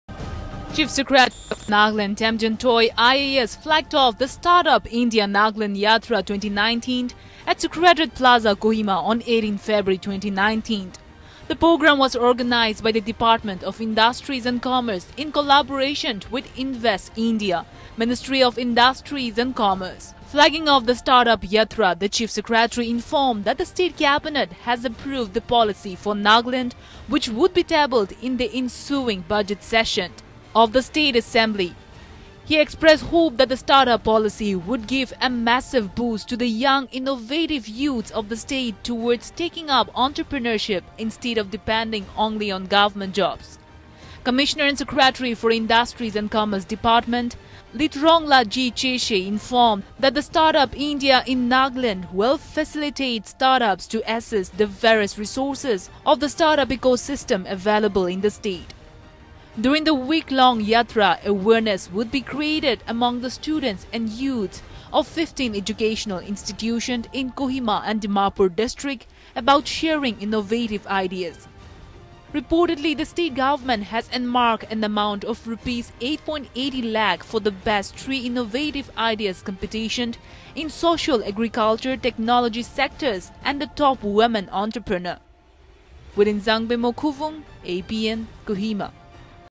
Nagaland CS flags off Startup India yatra in state; initiative to boost entrepreneurship
Startup India Nagaland Yatra 2019 kicked off with Chief Secretary of Nagaland Temjen Toy, IAS flagged off the startup at Secretariat plaza. The yatra is a flagship initiative to build a strong eco-system for nurturing innovation that will drive sustainable economic growth and generate large scale employment. The weeklong Yatra will culminate on 26th February.